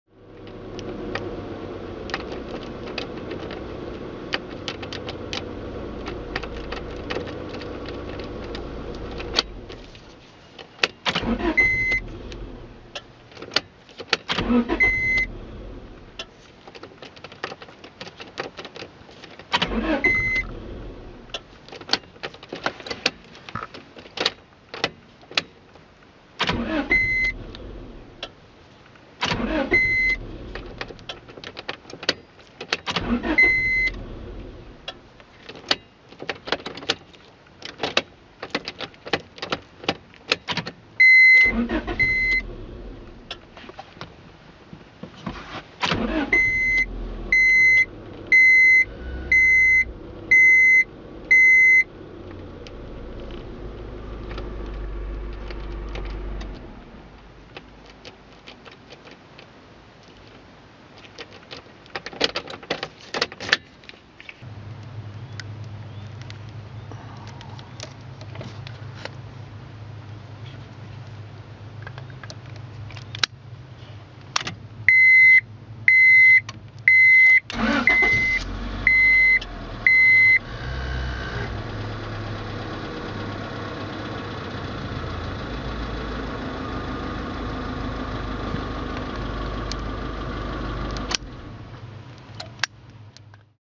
Checking Honda Odyssey Ignition Switch
The car dies while driving, starts right up, drives fine, then dies again, can't start... It's the ignition switch... For procedure go here